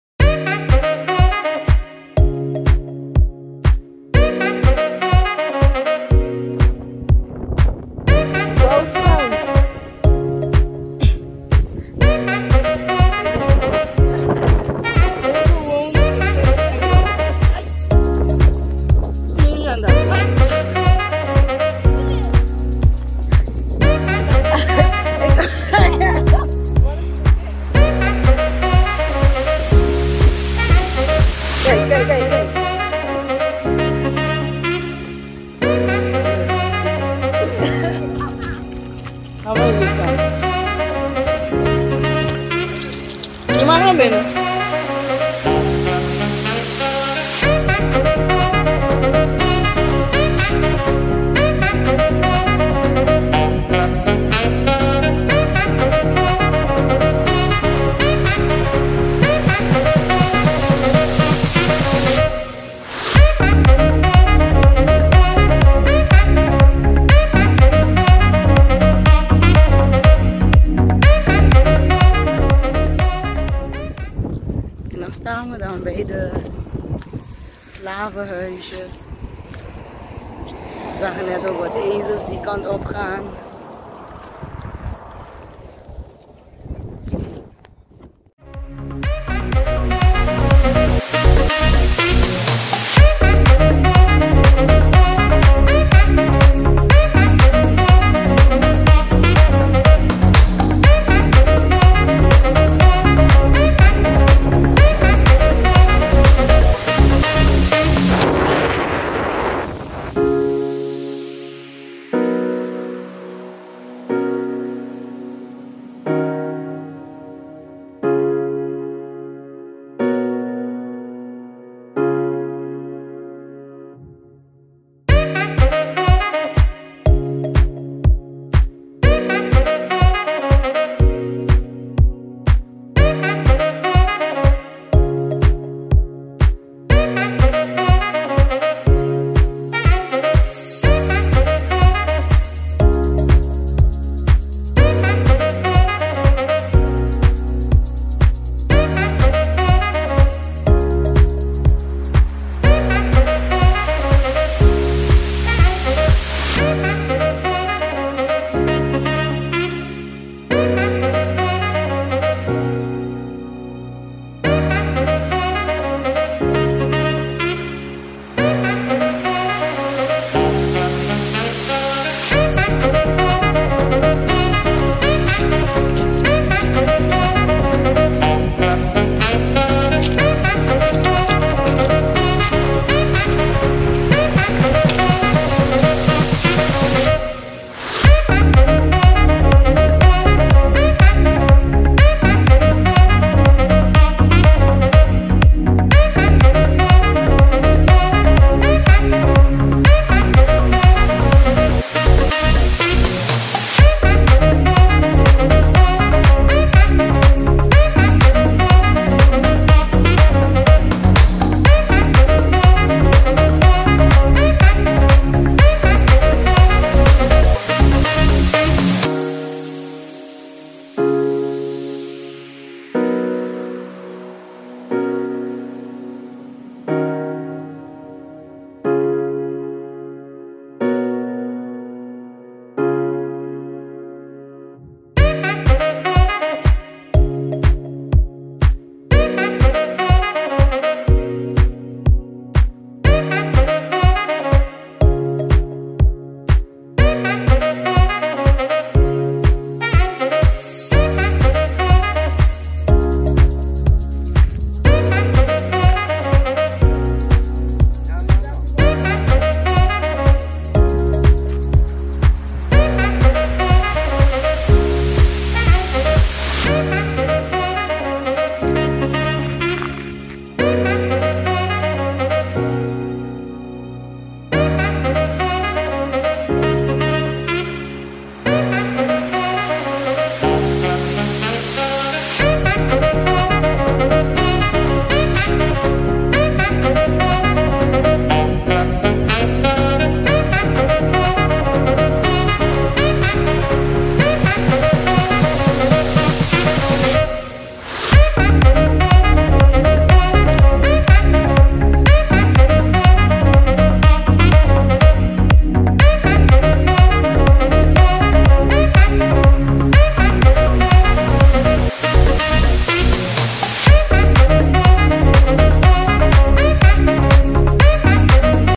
vakantie vlog # 3 - Bonaire rond trip!
Ik neem je mee  op een rond trip op de prachtige  eiland Bonaire!